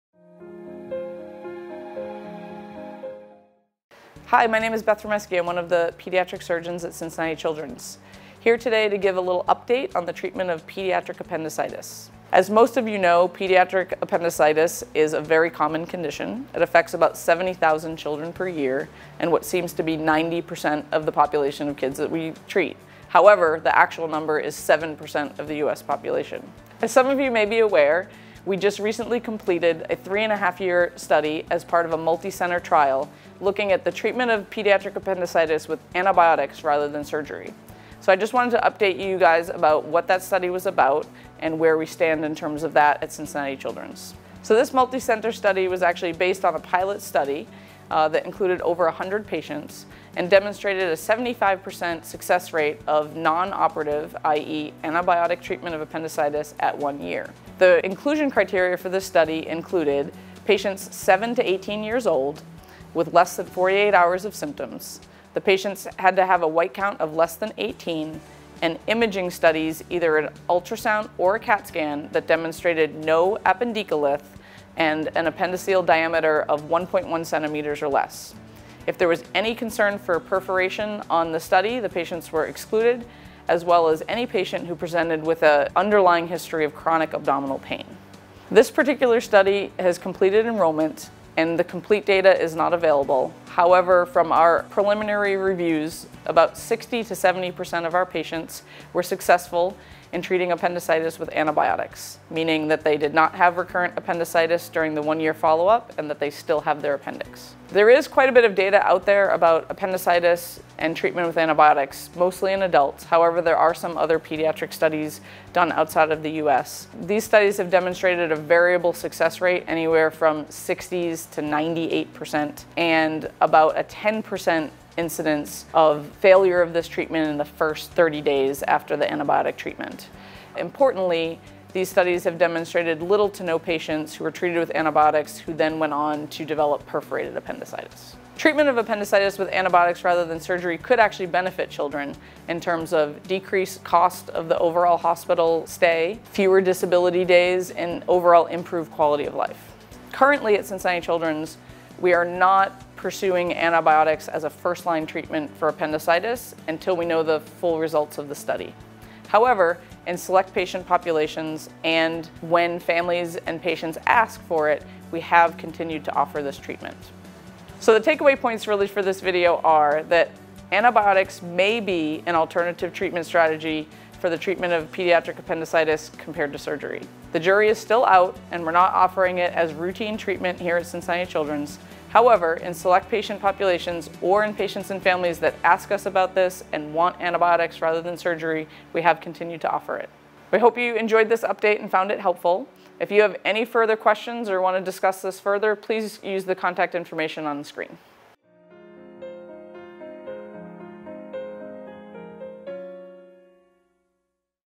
Treatment of Pediatric Appendicitis: Study Update | Cincinnati Children's
Beth Rymeski, DO:

Hi, my name is Beth Rymeski. I’m one of the pediatric surgeons at Cincinnati Children’s here today to give a little update about pediatric appendicitis.

As most of you know, pediatric appendicitis is a very common condition that affects about 70,000 children per year. And what seems to be about 90 percent of the population of kids we treat. However, the actual number is 7 percent of the U.S. population.

As some of you may be aware, we just recently completed a 3½-year study as part of a multi-center trial looking at the treatment of pediatric appendicitis with antibiotics rather than surgery. So, I just wanted to update you guys about what that study was about and where we stand in terms of that at Cincinnati Children’s.

So, this multi-center study was actually based on a pilot study that included over 100 patients and demonstrated a 75 percent success rate of non-operative I.e. antibiotic treatment of appendicitis at 1 year.

The inclusion criteria for this study included patients 7-18 years old with less than 48 hours of symptoms. The patients had to have a white count of less than 18. And imaging studies either and Ultrasound or a CAT scan that demonstrated no appendicolith and an appendiceal diameter of 1.1 centimeters or less. If there was any concern for perforation on the study the patients were excluded as well as any patient who presented with an underlying history of chronic abdominal pain.

This particular study has completed enrollment and the complete data is not available however from our from our preliminary reviews about 60-70% of our patients were successful in treating appendicitis with antibiotics. Meaning that they did not have recurrent appendicitis during the 1 year follow up and that they still have their appendix.

There is quite a bit of data out there about appendicitis and treatment with antibiotics, mostly in adults. However, there are some other pediatric studies done outside of the U.S. These studies have demonstrated a variable success rate anywhere from 60s to 98 percent. And about a 10 percent incidence of failure of this treatment in the first 30 days after the antibiotic treatment. Importantly these studies have demonstrated little to no patients, treated with antibiotics who then went on to develop perforated appendicitis.

Treatment of appendicitis with antibiotics rather than surgery could actually benefit children in terms of decreased cost of the overall hospital stay, fewer disability days and overall improved quality of life.

Currently, at Cincinnati Children’s, we are not pursuing antibiotics as a first-line treatment for appendicitis until we know the full results of the study. However, in select patient populations and when families and patients ask for it, we have continued to offer this treatment.

So, the takeaway points for at least this video are that antibiotics may be an alternative treatment strategy for the treatment of Pediatric Appendicitis compared to surgery. The jury is still out and we’re not offering it as routine treatment here at Cincinnati Children’s. However in select patient populations or in patients and families that ask us about this and want antibiotics rather than surgery we have continued to offer it.

We hope you enjoyed this update and found it helpful.